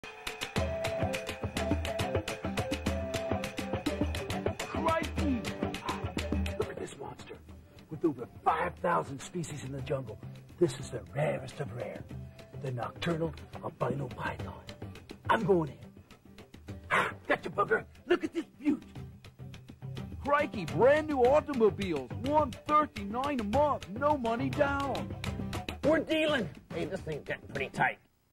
Ricart The Car Deal Hunter Commercial
Steve Irwin parody. From 2001